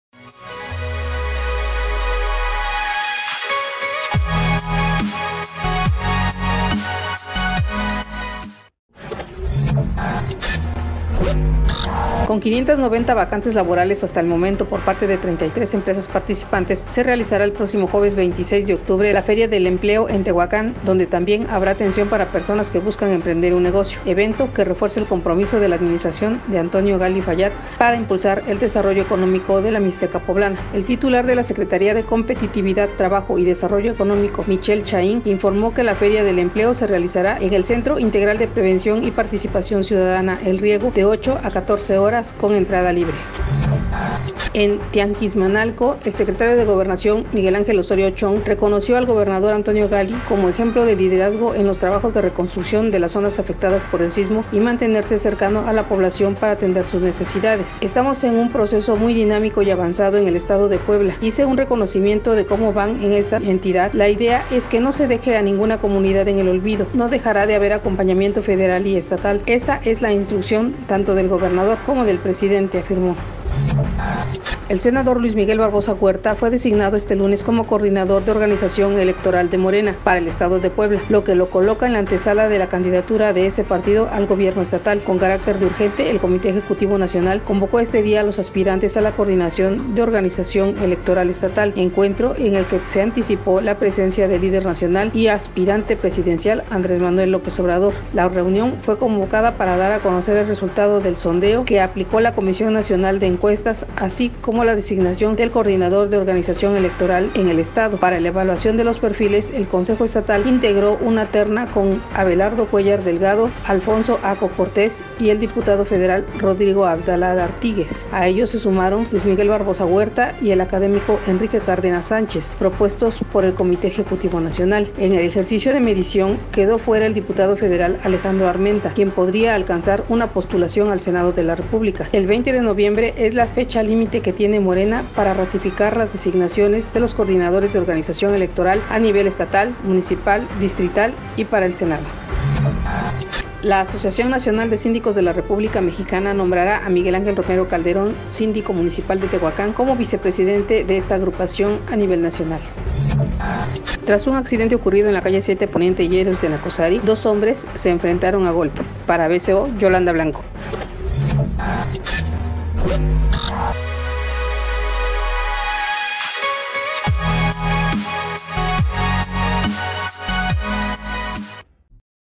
BCO TV El Uno 231017
BCO TV EL UNO 210917

GRACIAS POR VISITARNOS Y COMPARTIR

el próximo jueves 26 de octubre  se realizará la Feria de Empleo en Tehuacán, donde también habrá atención para personas que buscan emprender un negocio, evento que refuerza el compromiso de la administración de Antonio Gali para impulsar el desarrollo económico en la Mixteca poblana.

En Tianguismanalco, el Secretario de Gobernación, Miguel Ángel Osorio Chong, reconoció al gobernador Antonio  Gali como ejemplo de liderazgo en los trabajos de reconstrucción de las zonas afectadas por el sismo y por mantenerse cercano a la población para atender sus necesidades.

El senador Luis Miguel Barbosa Huerta fue designado este lunes como coordinador de organización electoral de Morena para el estado de Puebla, lo que lo coloca en la antesala de la candidatura de este partido al gobierno estatal.

.- La Asociación Nacional de Síndicos de la República Mexicana (ANSRM) nombrará y oficializará el cargo de vicepresidente de esa agrupación para el siguiente periodo lo asuma Miguel Ángel Romero Calderón Síndico de Tehuacán.

Dos hombres Se enfrentan a golpes tras  percance vial.